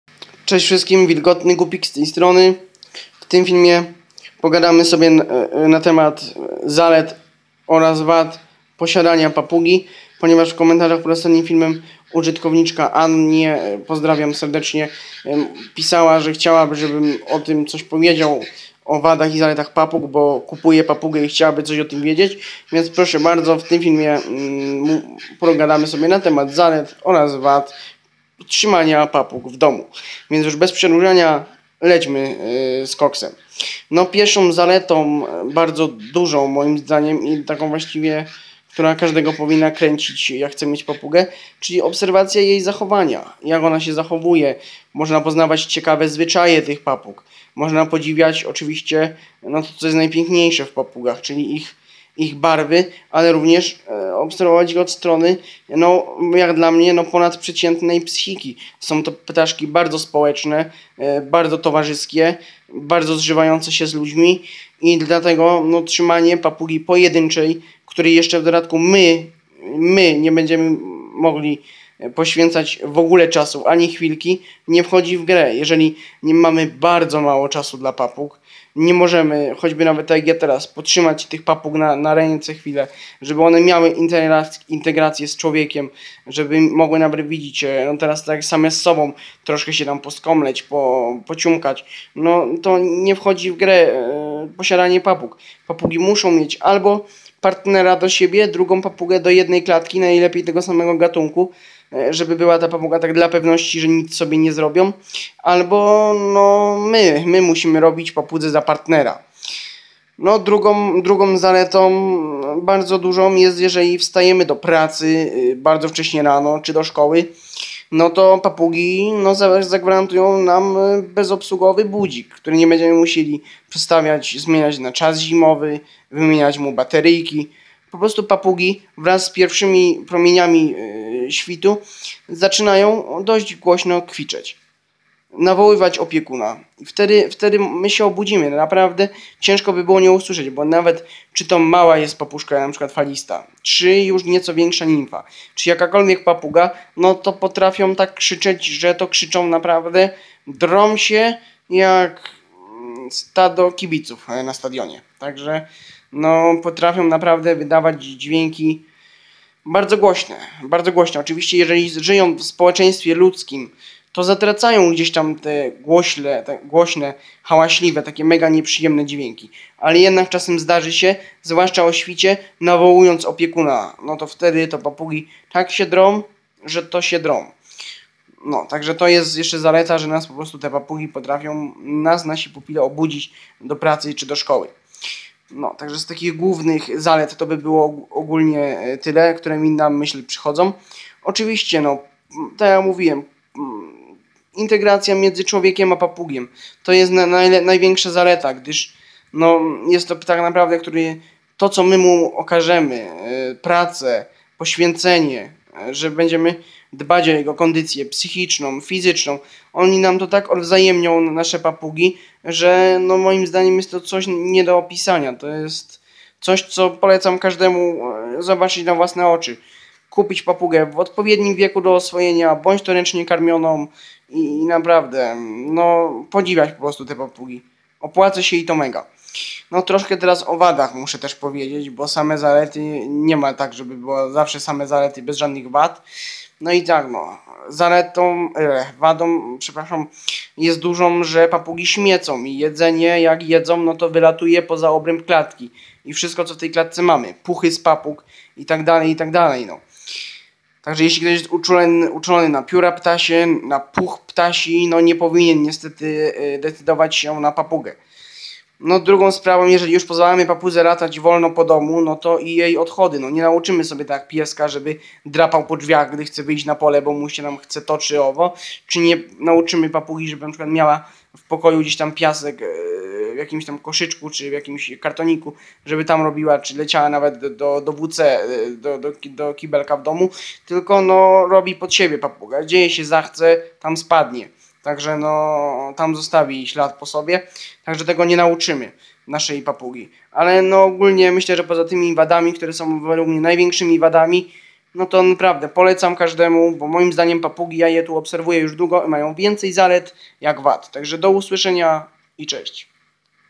Papugi w domu - czyli zalety i wady trzymania papug. (W opisie jeszcze dwie informacje o papugach).
Oczywiście jak zawsze kiedy nagrywam musi mi jakaś informacja z głowy wylecieć, więc tutaj ją daję. Papugi, które są bardzo zżyte emocjonalnie z ludźmi potrafią nawet nauczyć się naśladować dźwięki z otoczenia w tym też ludzką mowę, co oznacza iż papugę można nauczyć mówić. Mi osobiście jedną udało się nauczyć mówić i mówiła do mnie,, Cześć" zawsze jak widziała, że wchodzę do pokoju. Niestety pewnego dnia do domu mi wlazł kot i załatwił mi tą papugę. Była to papuga falista żółta o imieniu Carlos. Są filmy z Carlosem z początku mojego kanału, także chętntch zapraszam do ich obejrzenia.  Papugi niekiedy potrafią również karmić opiekuna przetrawionym pokarmem ze swojego wola. Jeśli papuga Cię karmi lub iska to znaczy, że darzy Cię największym stopniem zaufania i przyjaźni. Mój Carlos próbował karmić moje palce u rąk także był mega oswojony.  Możecie mi napisać, z czym chcielibyście kolejny materiał. Miłego oglądania.